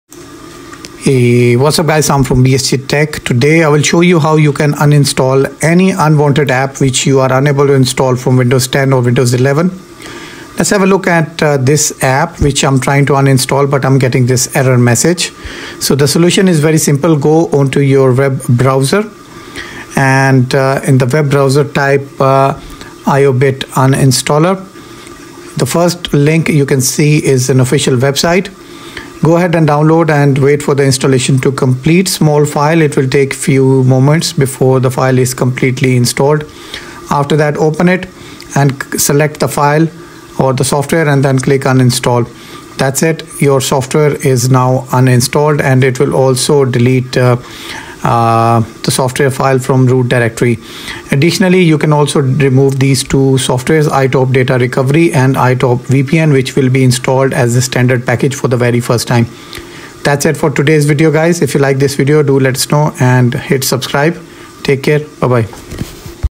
Can't Uninstall Program from windows 10 or Windows 11 ? Not A Problem
if you are unable to uninstall an app from windows 10 and windows 11, the solution is very simple

download IOBIT Uninstaller
run it and uninstall any apps with force
this method takes only few minutes and results will speak for its self

also , watch these videos for windows related solutions